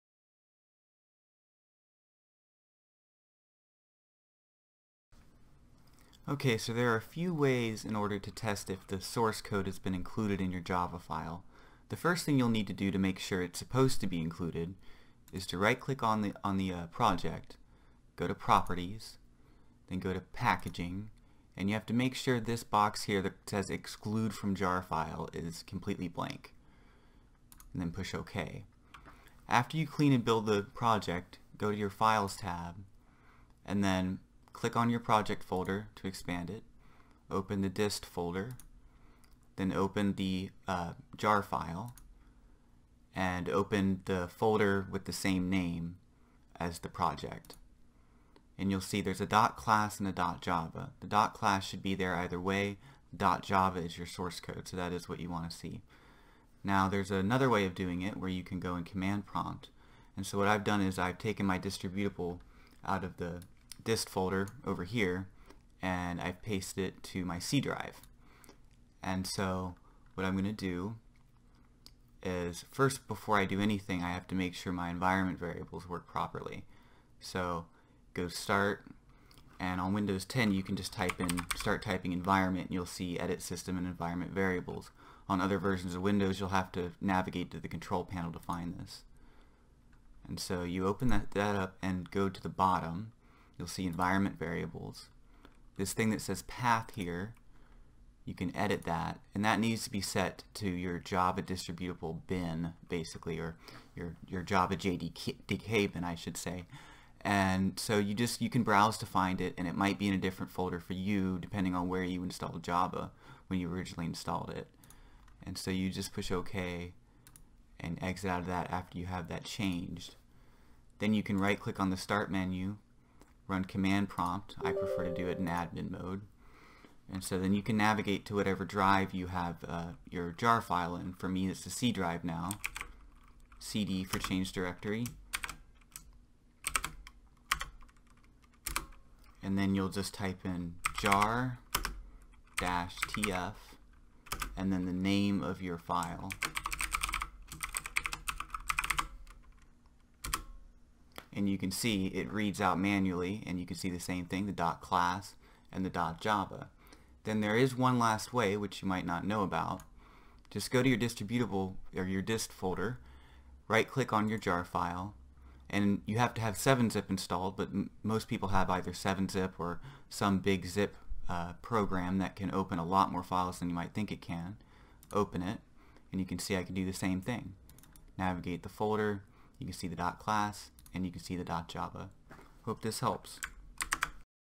How to Include Source Files in .Jar in Netbeans, and Ensure They are Being Included.
How to Include Source Files in .Jar in Netbeans, and Ensure They are Being Included. A lot of classes that teach Java may want you to include your source files with your submission. This is how to do that in Netbeans IDE and check for source files in any .Jar File.